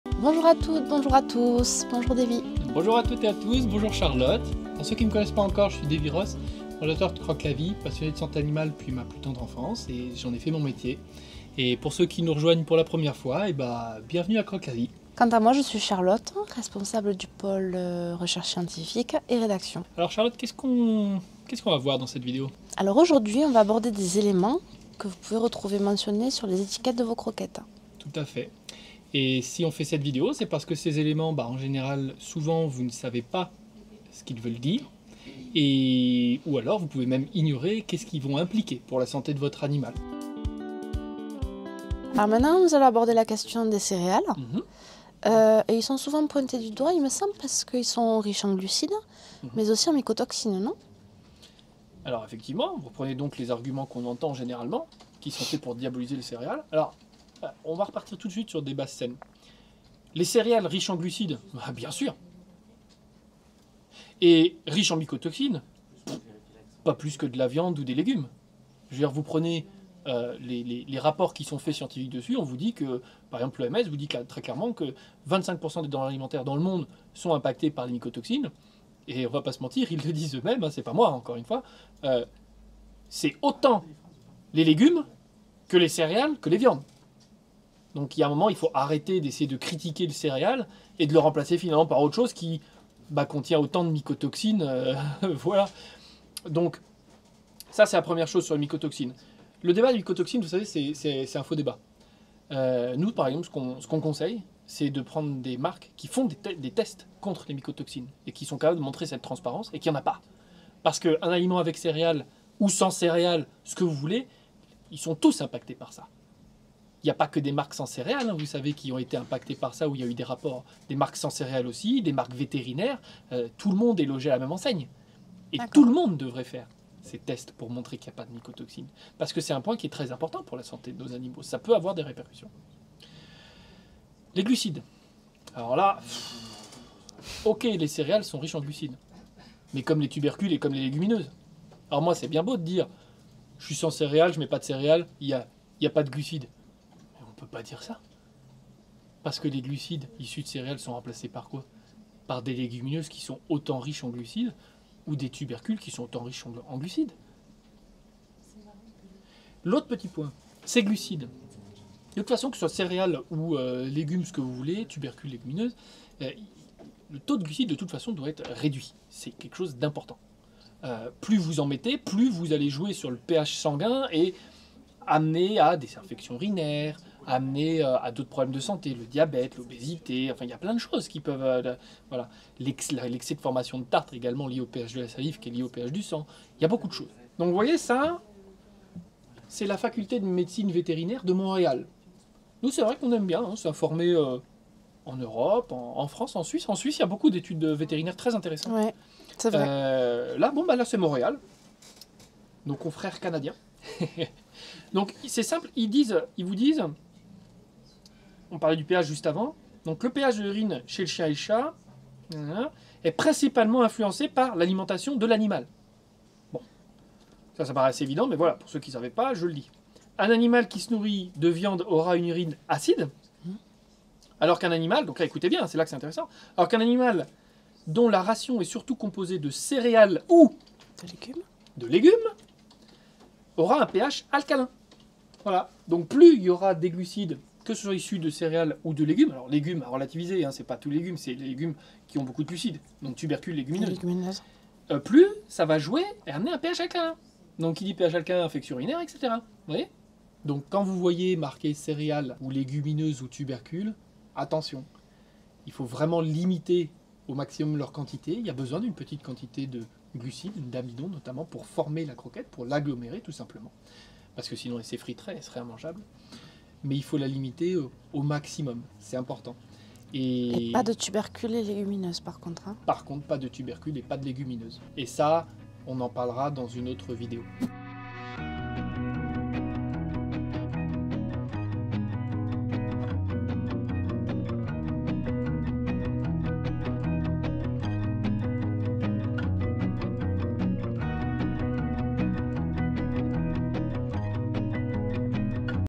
Les glucides dans les croquettes : BON ou MAUVAIS ?
Si vous avez le moindre doute sur la santé ou l'alimentation de votre animal, écrivez-nous de suite dans les commentaires.

- 00:00 : Introduction
- 00:45 : Les mycotoxines sont-elles que dans les céréales ?
- 01:40 : Céréales et Mycotoxine : un faux débat !
- 02:14 : Céréales, tubercules, légumineuses dans les croquettes
- 02:38 : Le taux de glucides doit être réduit pour la santé des chiens et chats

✅ Notre infographie sur les fondamentaux pour bien choisir vos croquettes (Profitez-en pour nous poser toutes vos questions par email)